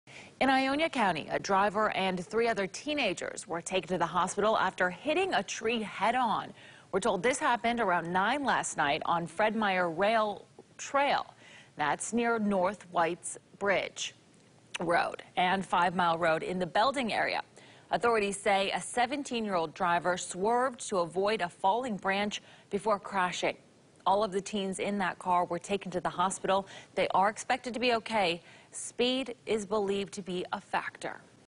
Crash injures 4 teens in Ionia
A driver and 3 other teenagers were taken to the hospital after hitting a tree head-on Saturday night.